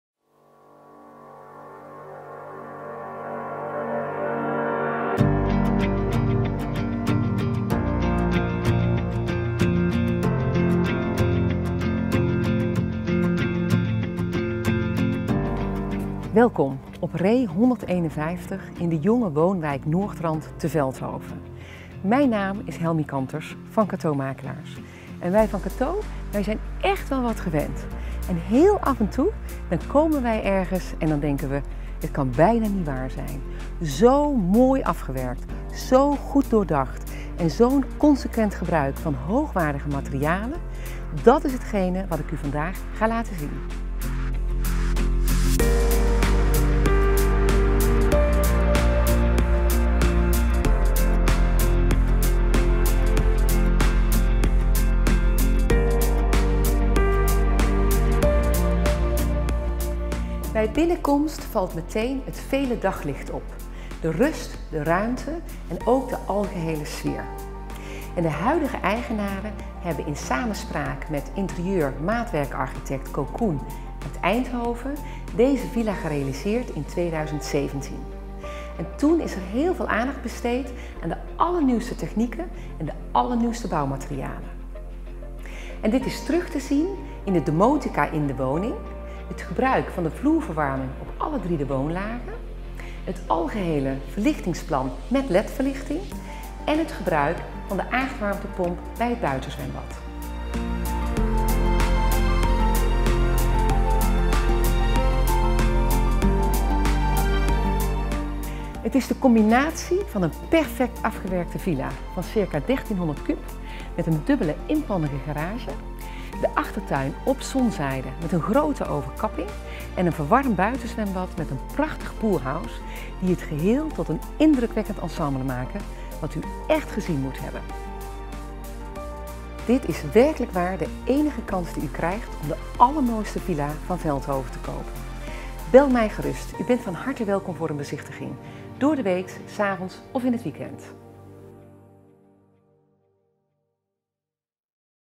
Ree 151, Veldhoven [verkocht] | Cato Makelaars Luxury Real Estate & Monuments
Bezoek onze website voor meer informatie: Cato Makelaars Luxury Real Estate and Monuments

6 pluspunten van de allermooiste familiewoning in Veldhoven en verre omgeving:

1. Deze topklasse villa (nieuwbouw 2018) met state-of-the-art techniek, wordt gekenmerkt door een ongekend en zeldzaam hoogwaardig afwerkingsniveau.

2. Symmetrie en harmonie, veel glas en heldere lijnen kenmerken de architectuur, met uitzonderlijk oog voor alle details, vol eigentijds comfort en technische hoogstandjes.

3. Ruimte, Ruimte, Ruimte: begane grond met huizenhoge ontvangsthal, werkkamer, woonkamer en leefkeuken circa 135 m² en garage voor twee auto's; vijf slaapkamers met garderobekamer, badkamer en waskamer op de verdieping. Circa 1300 m³ inhoud en circa 350 m² woonoppervlakte.

4. Luxe, Luxe, Luxe: Met circa 30 m² af te sluiten overkapping, met via aardwarmtepomp verwarmd buitenzwembad en exclusief pool house met badkamer alsmede magnifiek aangelegde achtertuin op zonzijde.

5. Gelegen in jonge woonwijk, de A2 en Eindhoven Airport bevinden zich op enkele minuten rijden. Limburg, de Randstad, België en Duitsland zijn snel en makkelijk bereikbaar.

6. Prijstechnisch zeer interessant voor degene die willen bouwen, gezien de prijsverhogende ontwikkelingen op de huidige nieuw te bouwen woningmarkt.